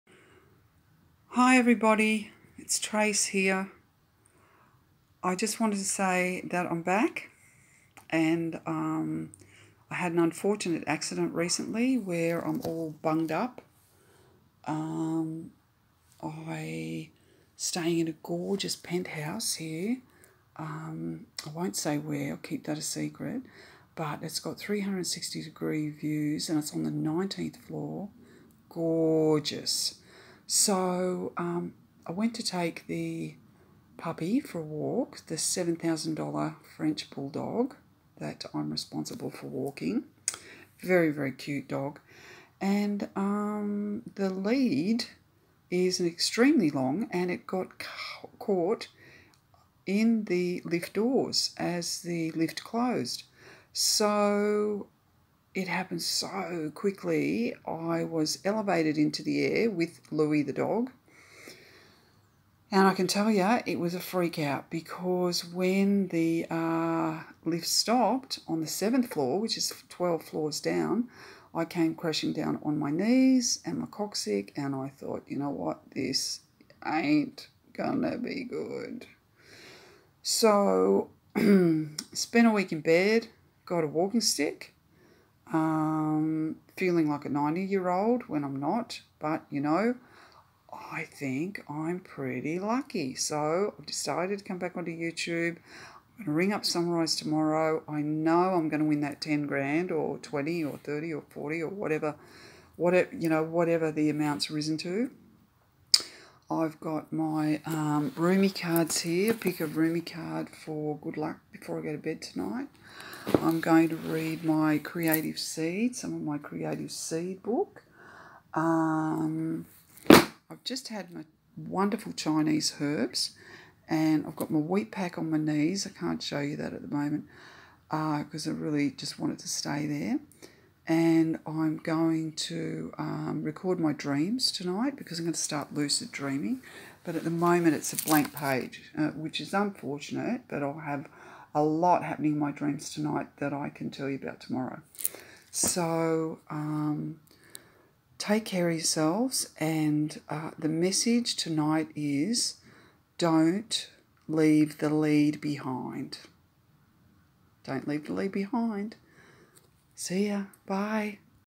Shower cap chat
What not to do.when living on the 19th floor and leaving half the dog lead in the lift as it closes!!!